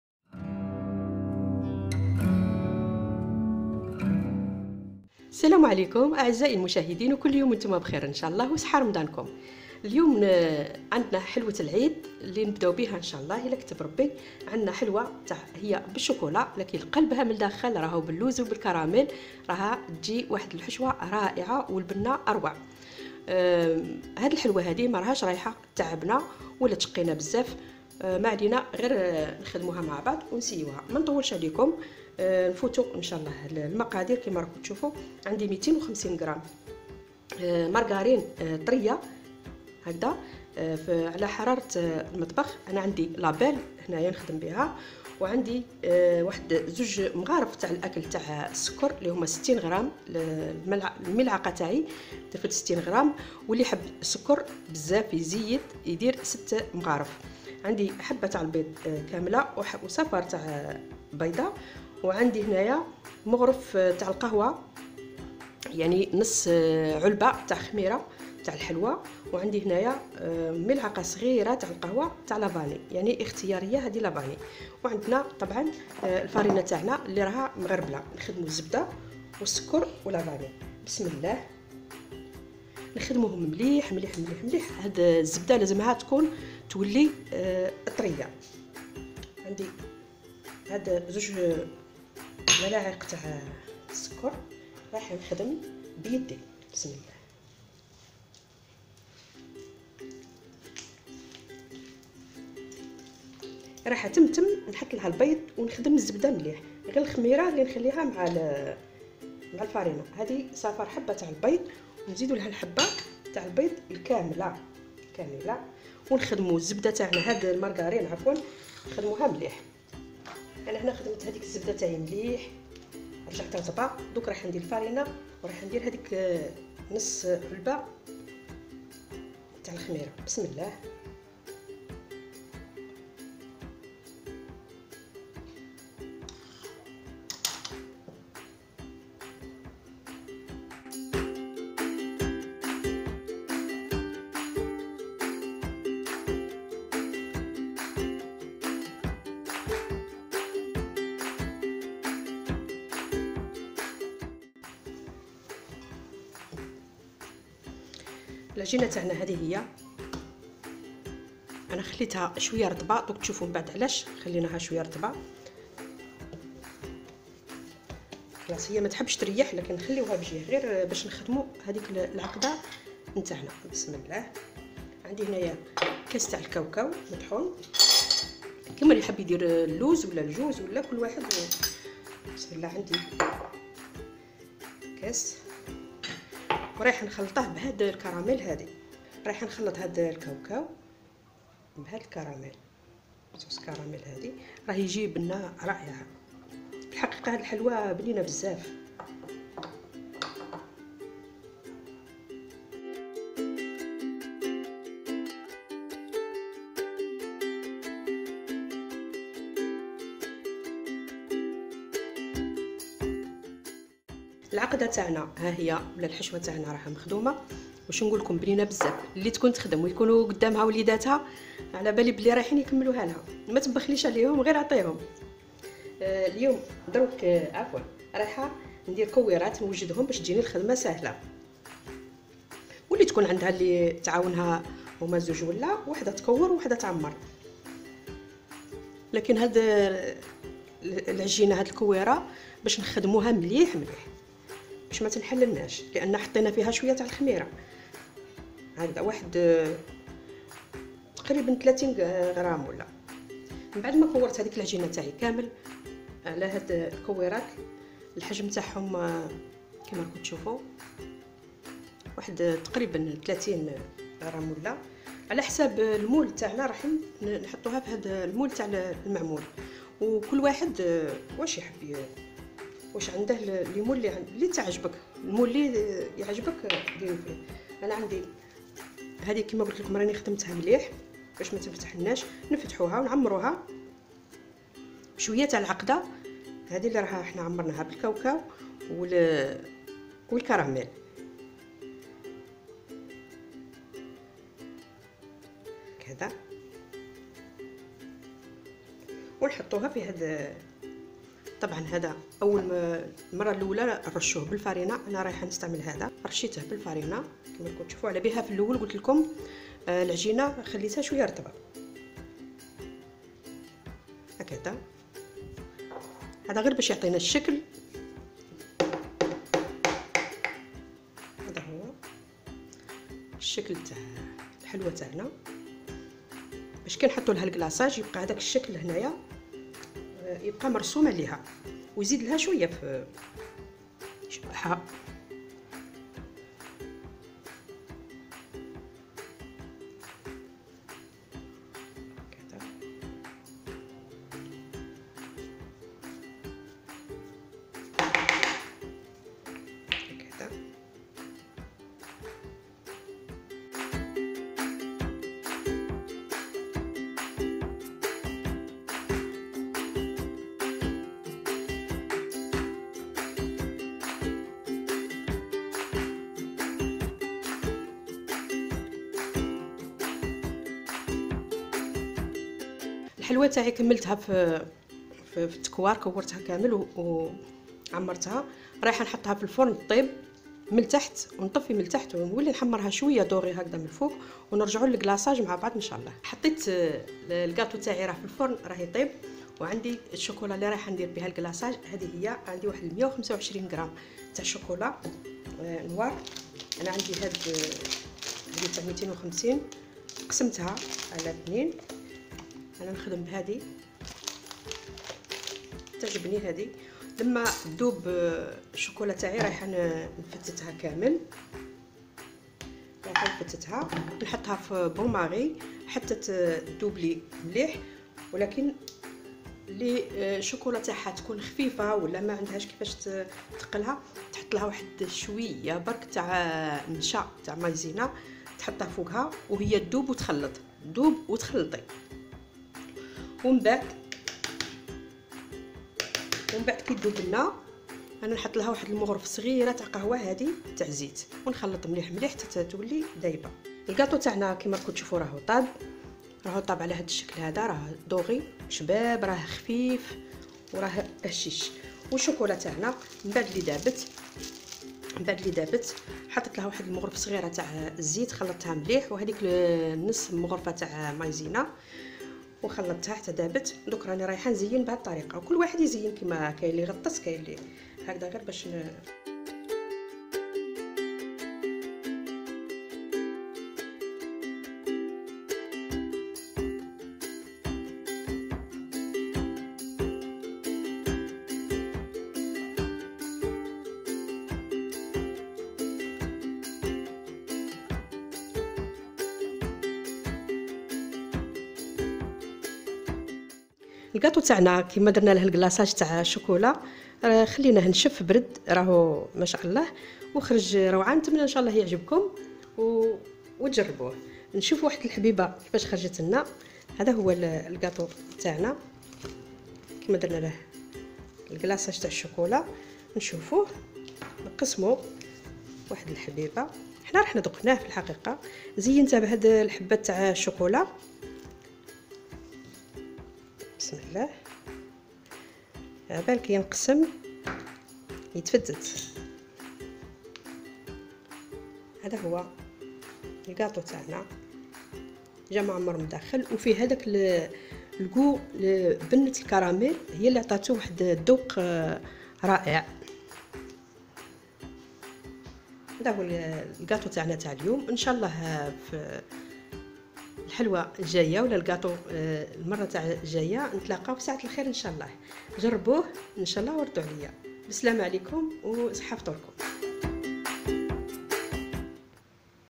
حلوة العيد محشية بالكوكاو والكراميل | مطبخ زهرة علوش
الوصفة↓↓↓
250غ مرغرين طرية
2 ملاعق كبار سكر
بيضة كاملة + صفار بيضة
نصف كيس خميرة
ملعقة صغيرة لفاني
فرينة
الحشو ↓↓↓
كاس كوكاو مرحي نخلطوه بالكراميل
تزيين ↓↓↓
شوكلاطة سوداء